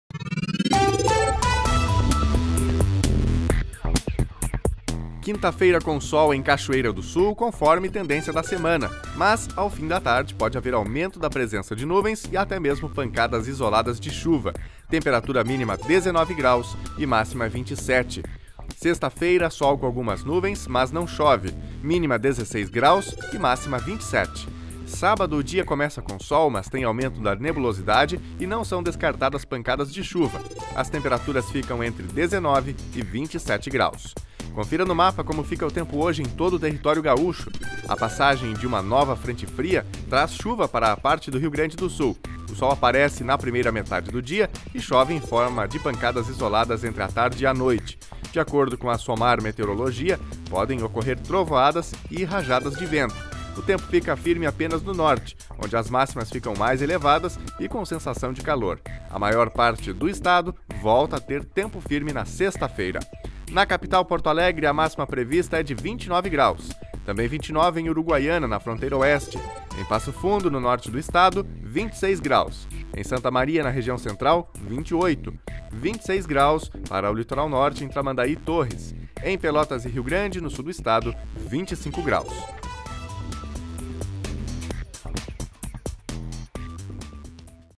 Frente fria causa alterações no tempo
Veja a previsão do tempo para Cachoeira do Sul e para todo o RS.